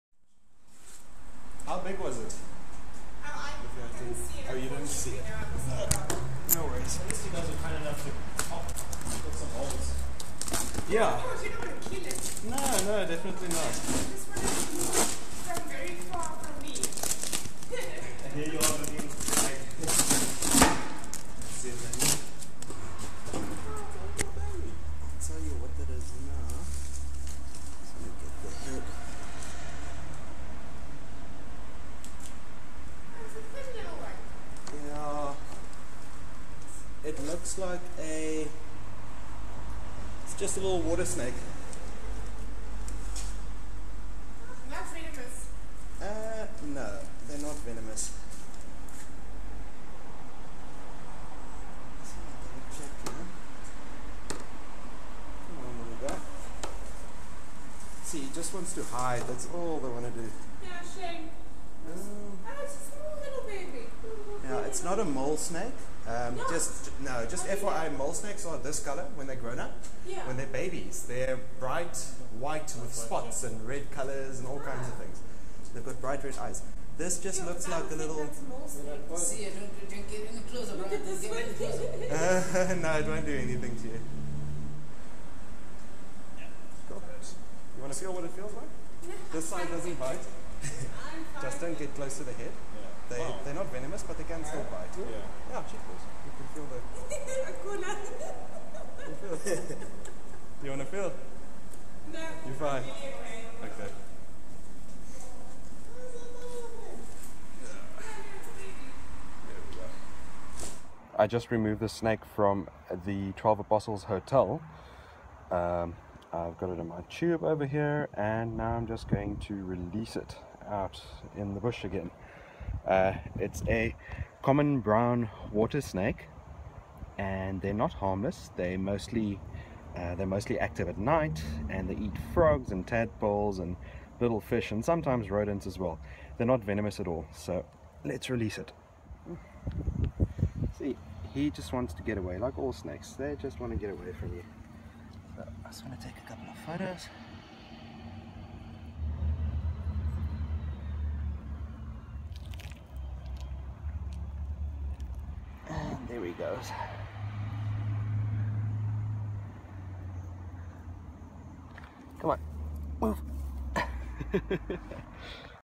Olive House Snake near Camps Bay, Cape Town, Western Cape, South Africa (20161221)
Olive House Snake (Lamprophis inornatus) - non-venomous and harmless to humans.

CORRECTION: I incorrectly identified this snake as a "Common Brown Water Snake (Lycodonomorphus rufulus) in the video.

I was called out to remove a small unidentified snake from Camps Bay today. It turned out to be an Olive House Snake - these guys are nocturnal and tend to be a uniform olive green, olive gray, light brown, or brownish black to black in color, with underparts uniformly dark as above or slightly lighter.

They are oviparous, laying 5 - 15 eggs in summer.

Even tiny amounts from enough of our fans would help us cover our fuel cost & time - simply click on one of the "Donate" buttons